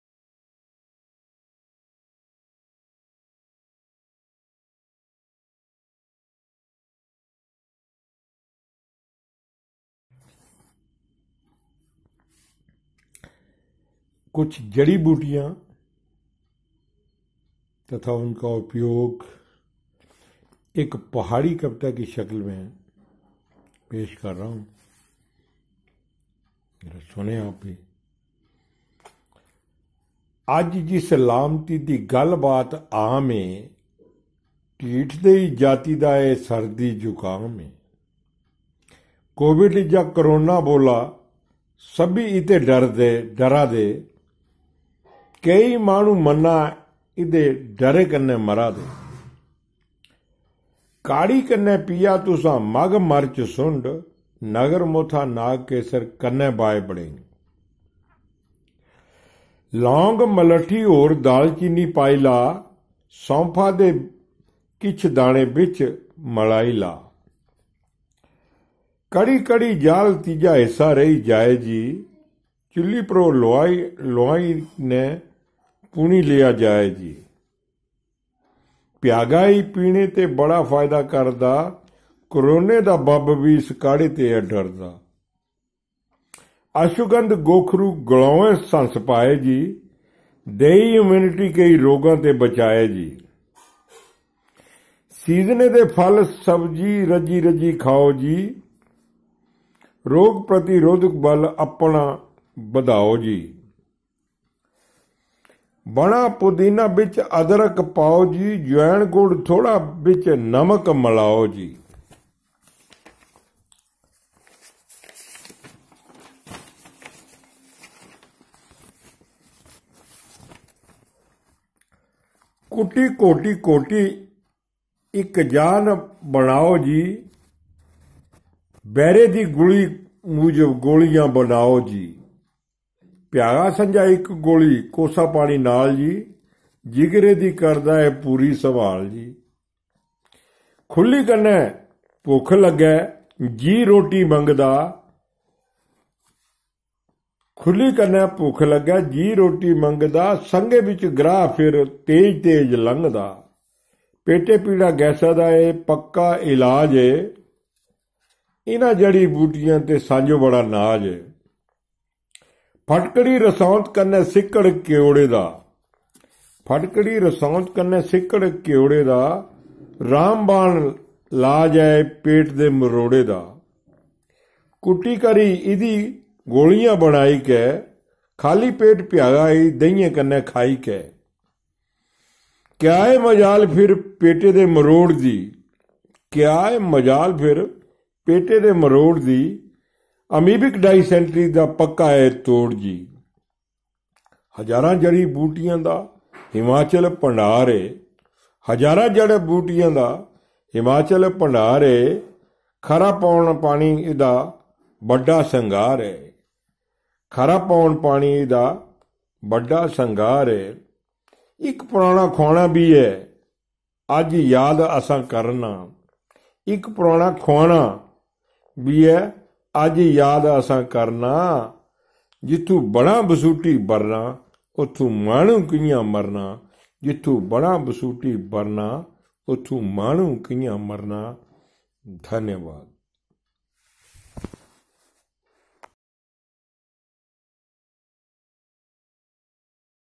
Herbs and Their Uses | Himachali Poem | कुछ औषधीय जड़ी बूटियां एवं उनका उपयोग | पहाड़ी कविता
कुछ औषधीय जड़ी बूटियां एवं उनका उपयोग | पहाड़ी कविता | कुंदन लाल भारद्वाज

अज्ज जिस लामती दी गल्लबात आम ऐ,
ढीठ देही जाति दा एह सर्दी ज़ुकाम है।
कोबिड जां करोना बोला सब्बी इहते डरा दे,
क‌ई माहणू मन्ना इहदे डरे कन्ने मरा दे।।

काहढ़ी कन्नै पिया तुसां मग,मर्च,सुण्ड,
नगरमोथा नागकेसर कन्नै बायबड़िंग।
लौंग,मलट्ठी होर दालचीनी पाई ला ।
सौंफा दे किच्छ दाणे विच्च मल़ाई ला।।
कहढ़ी कहढ़ी जाहल तीज्जा हिस्सा रही जाए जी,
चुल्ही प्रो लुआही नै पुणी लिया जाए जी।
भ्यागा ई पीणे ते बड़ा फायदा करदा,
करोने दा बब्ब बी इस काहढ़े ते ऐ डरदा।।

अश्वगन्ध गोखरु गल़ौंएं सहंसपाए जी,
देई इम्युनिटी क‌ई रोगां ते बचाए जी।
सीज़ने दे फ़ल़ सब्जी रज्जी रज्जी खाओ जी,
रोग प्रतिरोधक बल अपणा बधाओ जी।।

बणाह पुदीना बिच्च अदरक पाओ जी
ज्वैण गुड़ थोड़ा बिच्च नमक मल़ाओ जी।
कुट्टी घोटी घोटी इकजान बणाओ जी
बैरे दी गुल़ी मूजव गोलियां बनाओ जी।।
भ्यागा संज्जा इक गोल़ी कोसा पाणी नाल़ जी,
जिगरे दी करदा एह पूरी सम्भाल़ जी।
खुलही कन्नै भुक्ख लग्गै, जी रोटी मंगदा,
संग्घे बिच्च ग्राह फिरी तेज तेज लंगदा।।
पेटे पीड़ा,गैसा दा एह पक्का इलाज ऐ
इन्हां जड़ी बूटियां ते सांजो बड़ा नाज़ ऐ।।

फटकड़ी रसौंत कन्नै सिक्कड़ क्योड़े था,
रामबाण लाज ऐ पेटे दे मरोड़े दा।
कुट्टी करी इहदी गोल़ियां बनाई कै,
खाली पेट भ्यागा ई दहिंयें कन्नै खाई कै।।
क्या ऐ मजाल फिरी पेटे दे मरोड़ दी,
अमीविक डाईसैंटरी दा पक्का एह तोड़ जी।

हजारां जड़ी बूटियां दा हिमाचल भंडार ऐ,
खरा पौण पाणी इहदा बड्डा संगार ऐ।

इक पुराणा खुआणा बी ऐ अज्ज याद असां करना,
"जित्थूं बणाह बसूटी बरना ओत्थूं माहणू किञा मरना"।।
जित्थूं बणाह बसूटी बरना. "

धन्यवाद। कुंदन लाल भारद्वाज